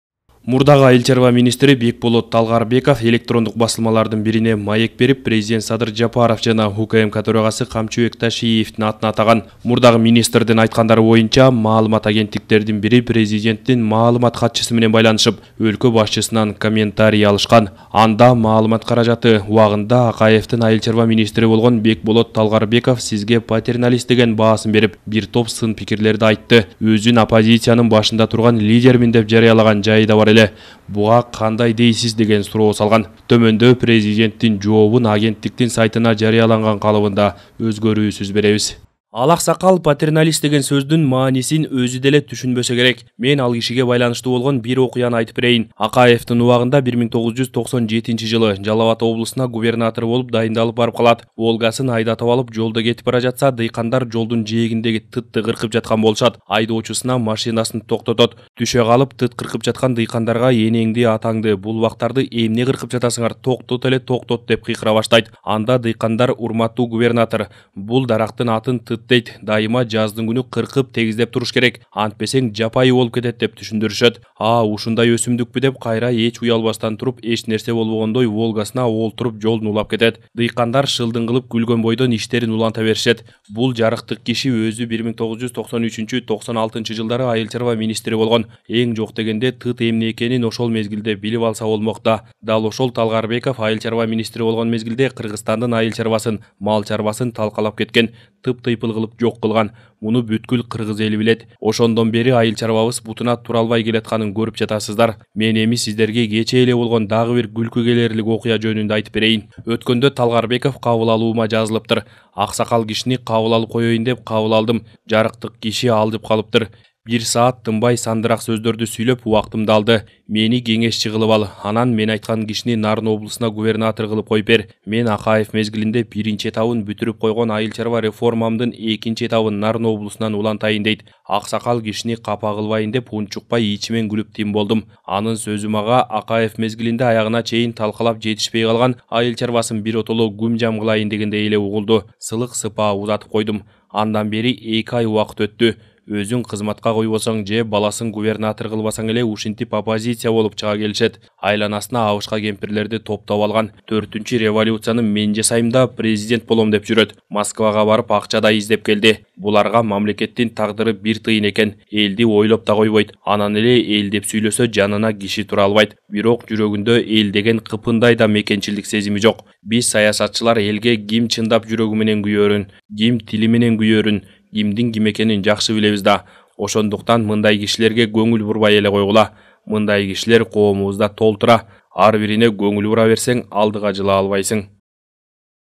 Президент Талгарбековдун маегине карата жооп кайтарды
Мурдагы Айыл чарба министри Бекболот Талгарбеков электрондук басылмалардын бирине маек берип, Президент Садыр Жапаров жана УКМК төрагасы Камчыбек Ташиевдин атын атаган. Мурдагы министрдин айткандары боюнча маалымат агеттиктердин бири президенттин маалымат катчысы менен байланышып, өлкө башчысынан комментарий алышкан. Анда маалымат каражаты: "Убагында Акаевдин айыл чарба министри болгон Бекболот Талгарбеков сизге патерналист деген баасын берип, бир топ сын-пикирлерди айтты. Өзүн оппозициянын башында турган лидермин деп жарыялаган жайы да бар эле. Буга кандай дейсиз?", - деген суроо салган. Төмөндө Президенттин жообун агенттиктин сайтына жарыяланган калыбында өзгөрүүсүз беребиз.